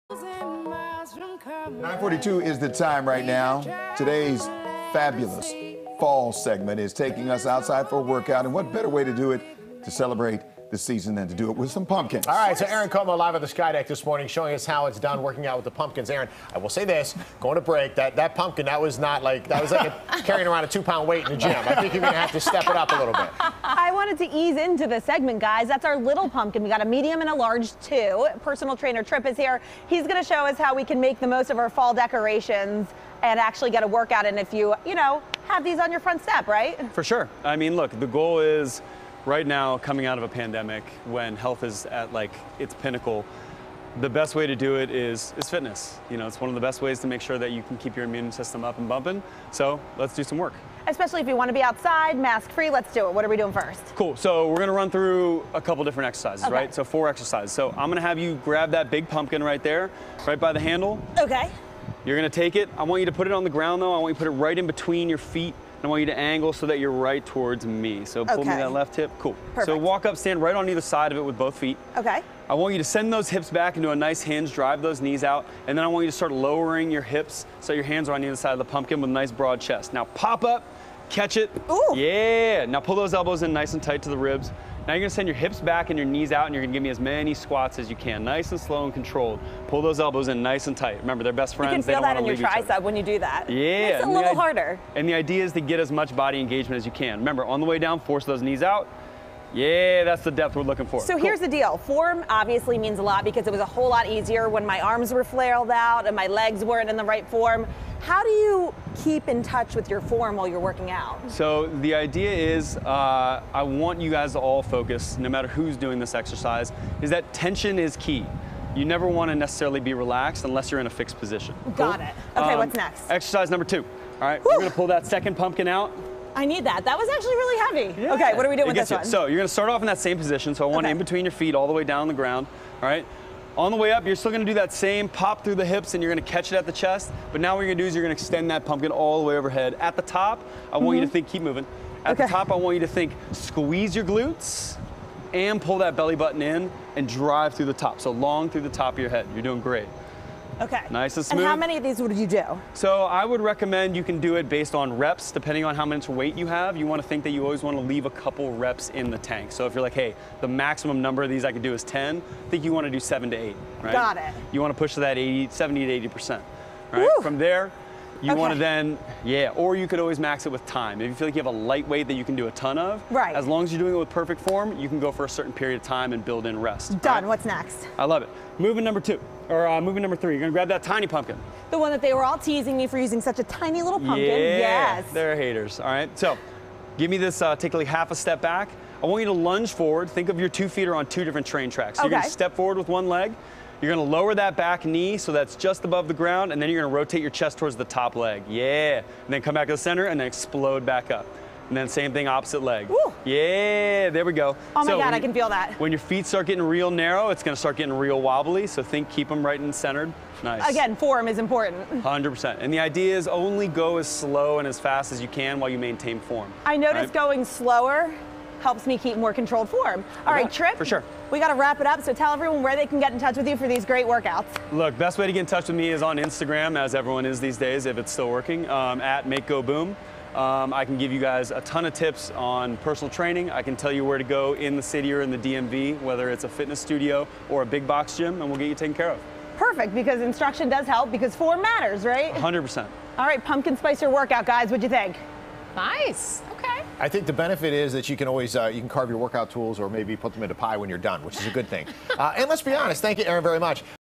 You can workout with...pumpkins?! YES | FOX 5 DC
Looking to (pumpkin) spice up your workout routine?! FOX 5's Erin Como tries out some GOURD-ous moves with personal trainer Trip O'Connell.